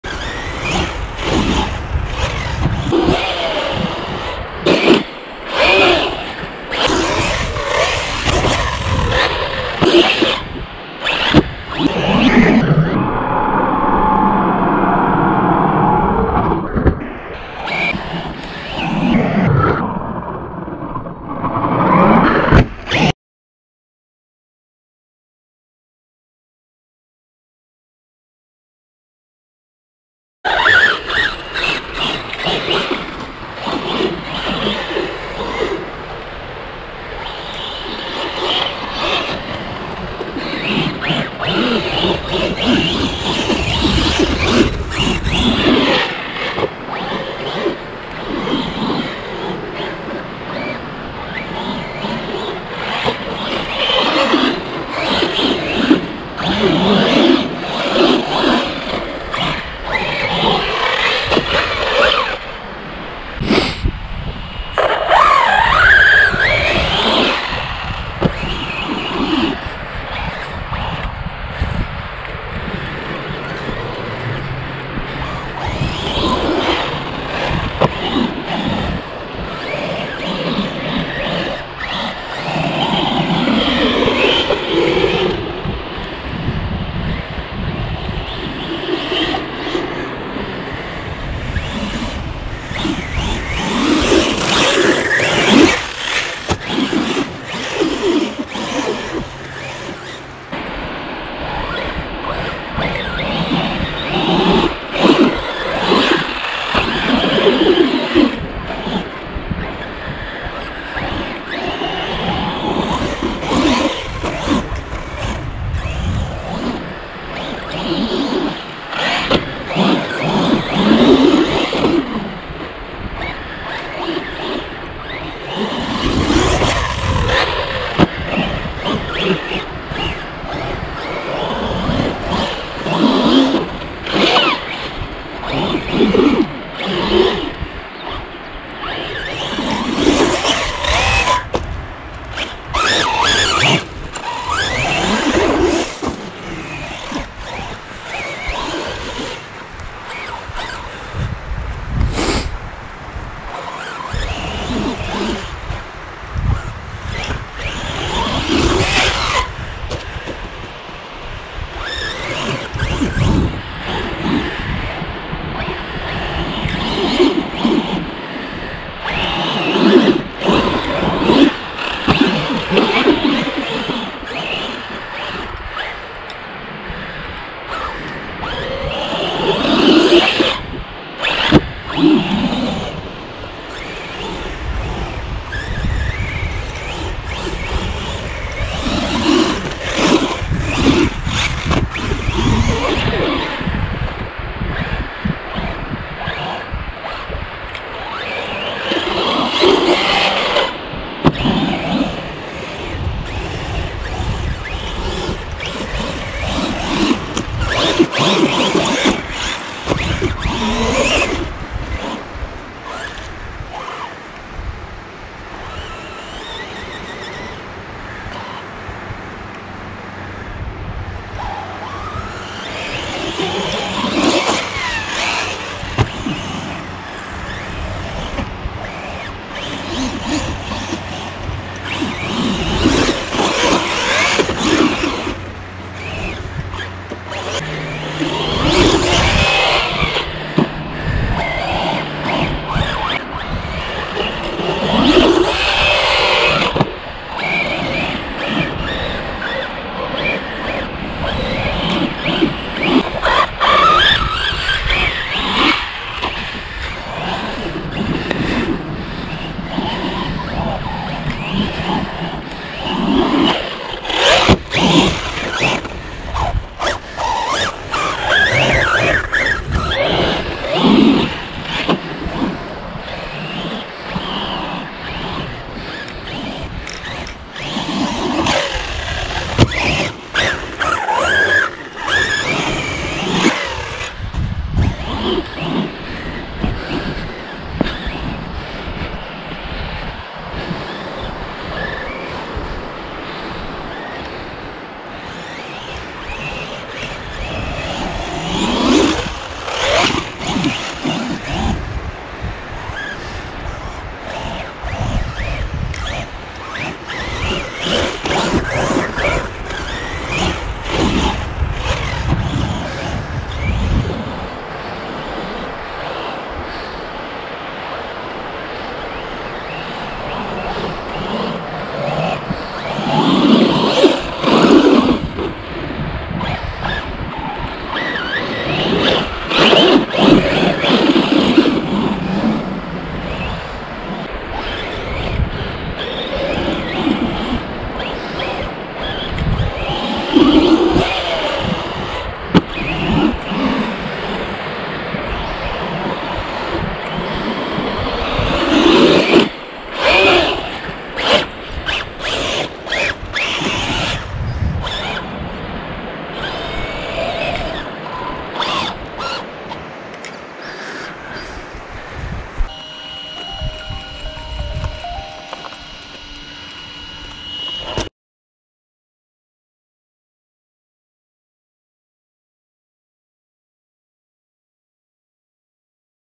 Corally asuga 6s roue paddle 🤘
Corally asuga 6s
Modification : tourelle et châssis alu 70/75 t6 ADU racing
           huile de différentiel av 100k cen 100k arr 50k
Roue paddle proline dumont